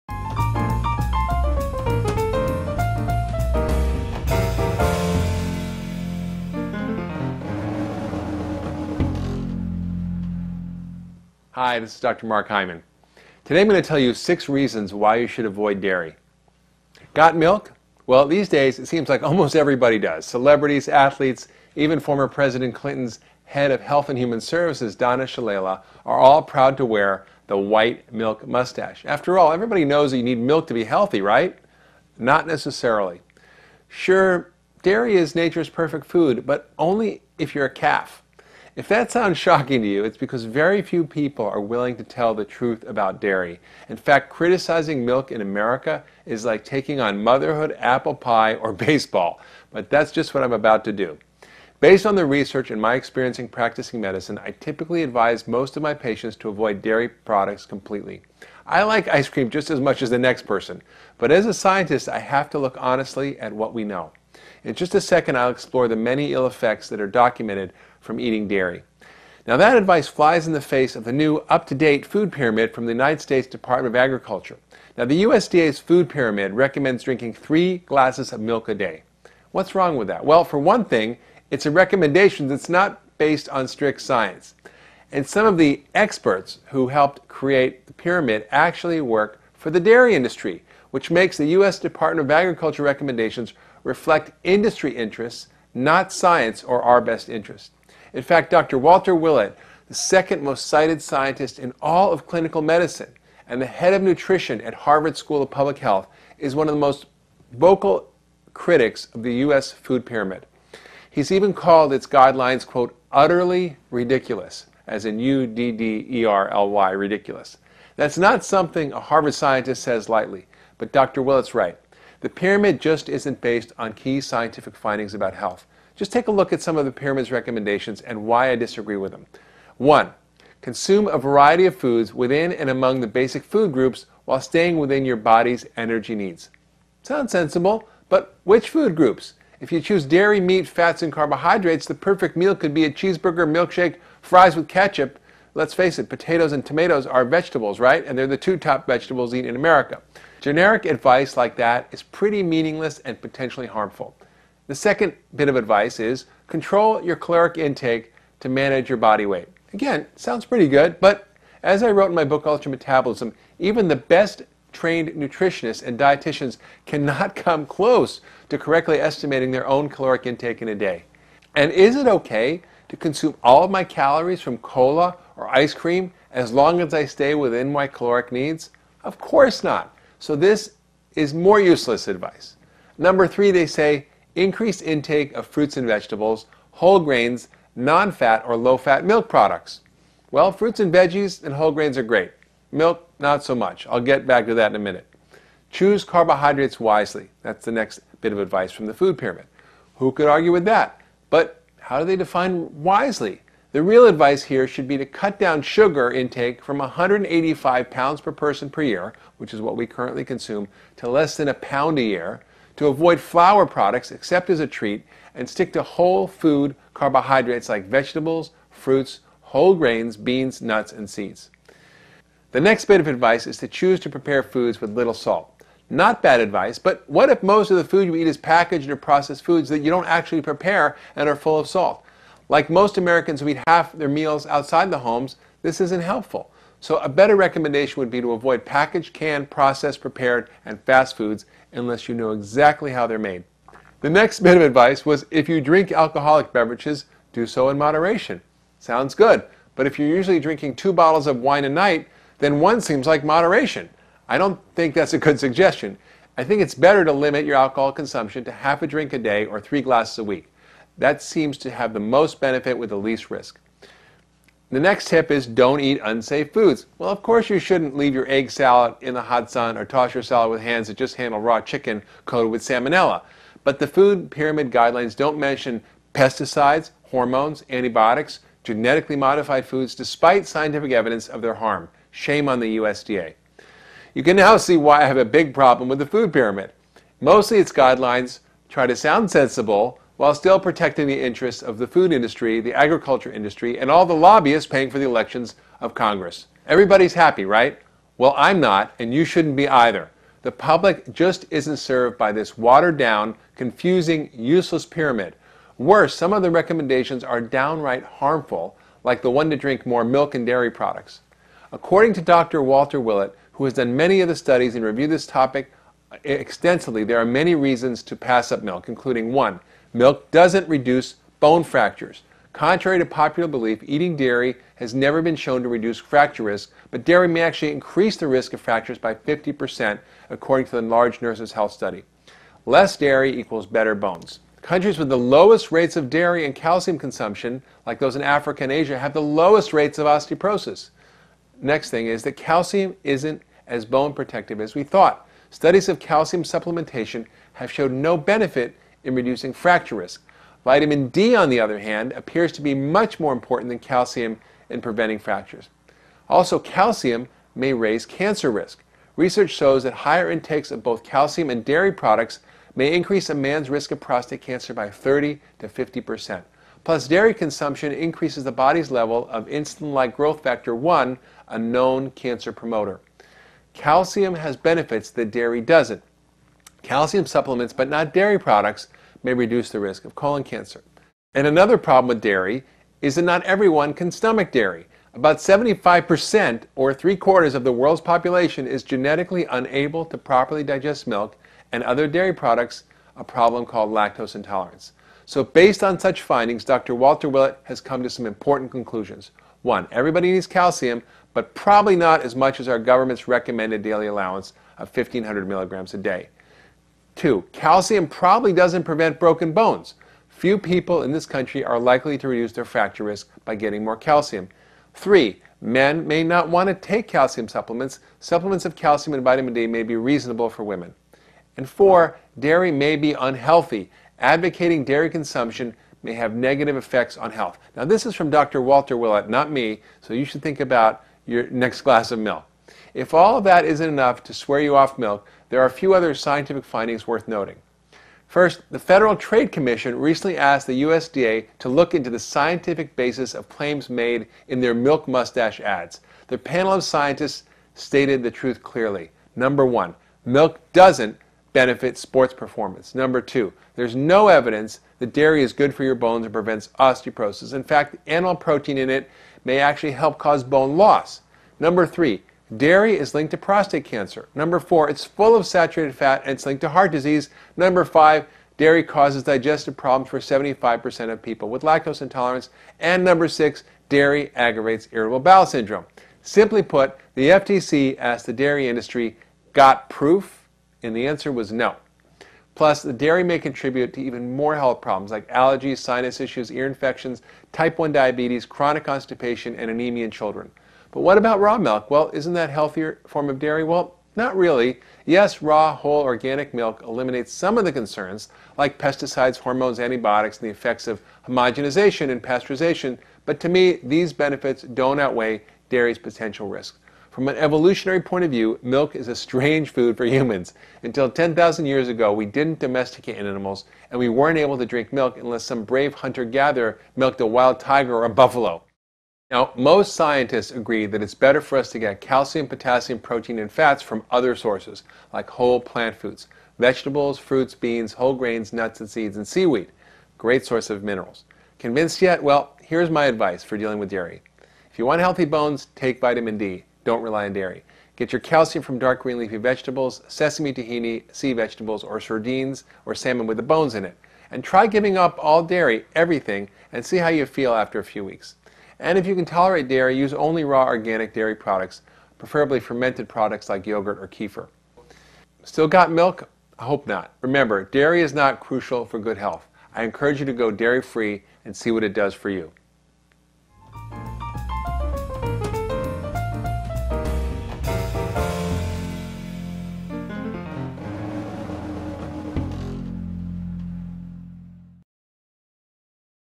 Dairy: 6 Reasons You Should Avoid It at all Costs
Got milk? Plenty of people think its perfectly healthy to drink, and advertisements would have you eating dairy all the time. But it may not be as healthy as you think. In this weeks UltraWellness blog Dr. Mark Hyman gives six reasons you should avoid milk and explains why it may be at the very root of your health problems.